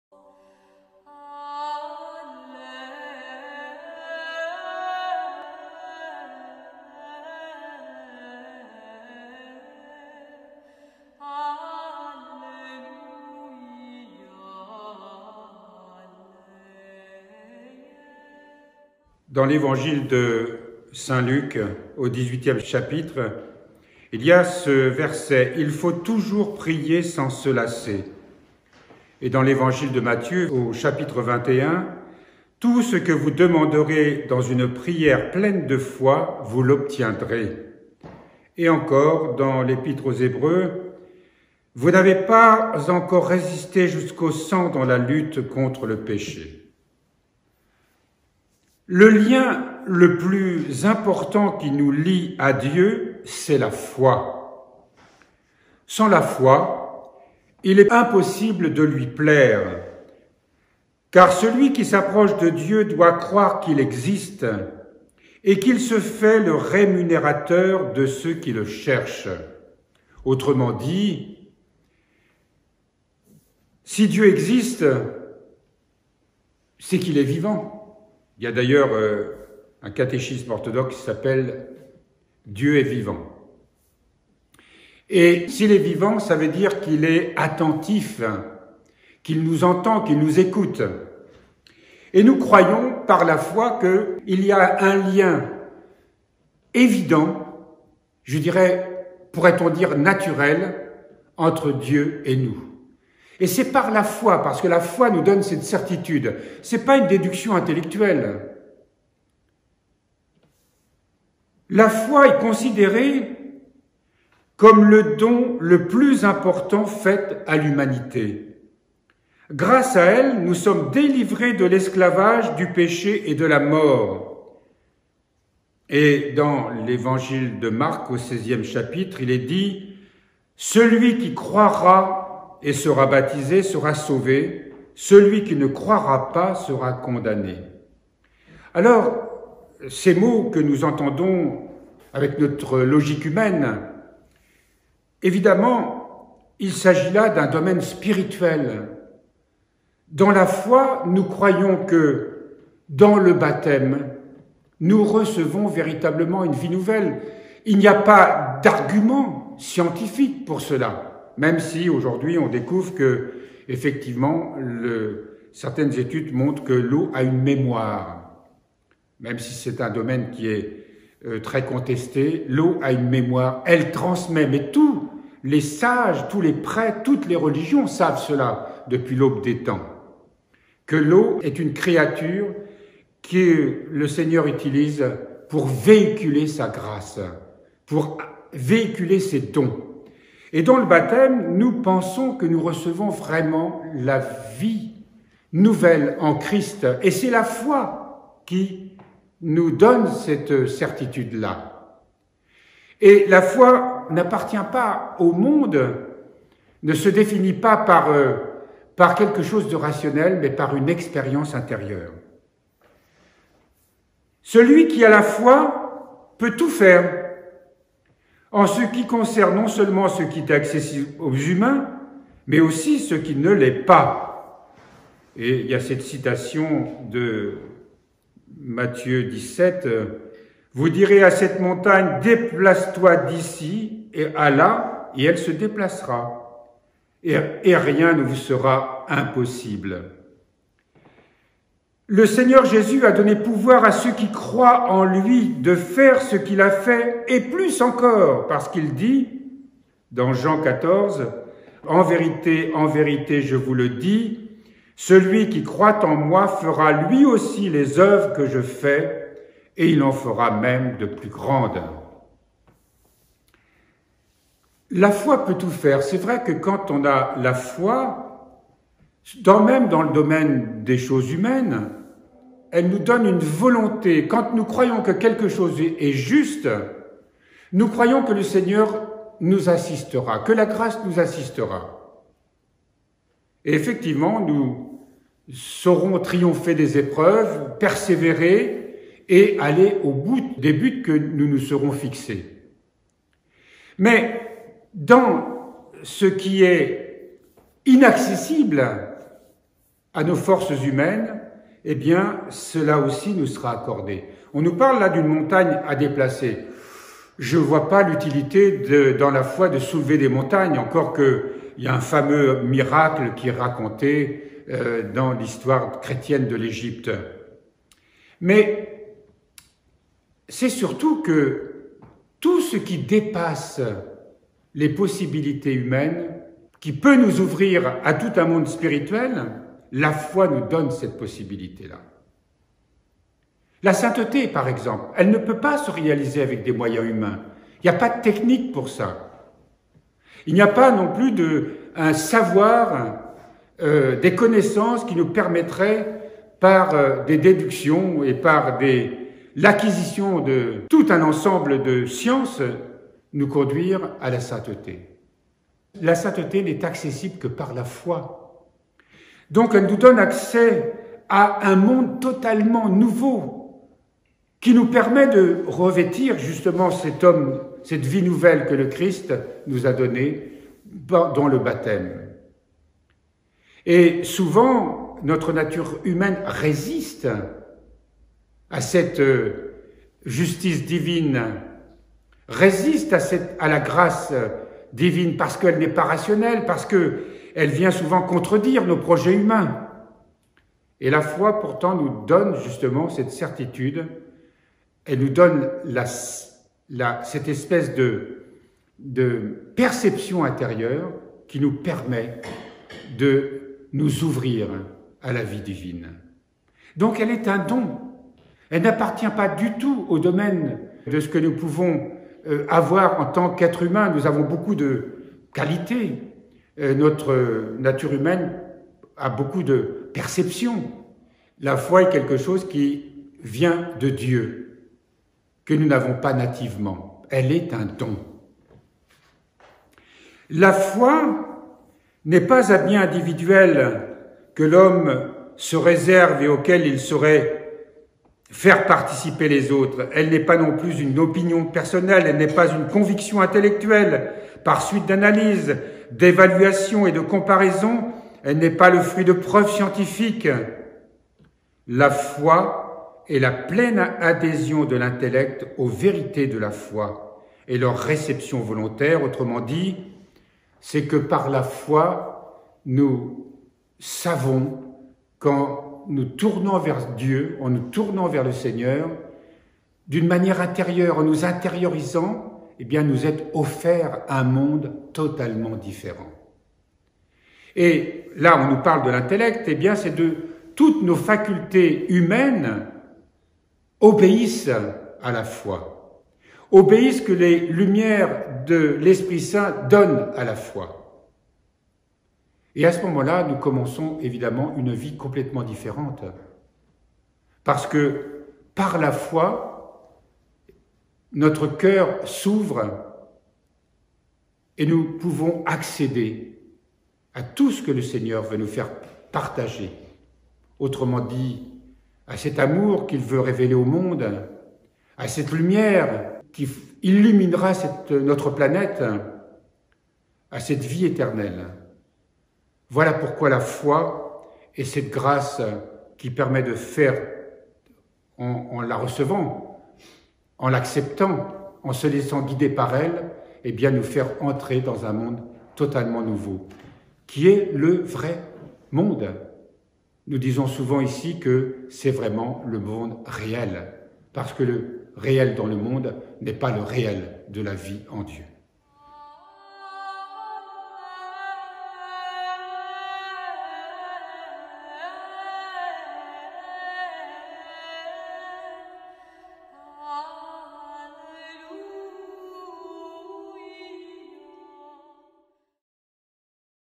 Foi et persévérance - 1
Méditation de Mgr Marc, tirée du livre : L' expérience de Dieu dans la vie de prière, du père Matta El-Maskîne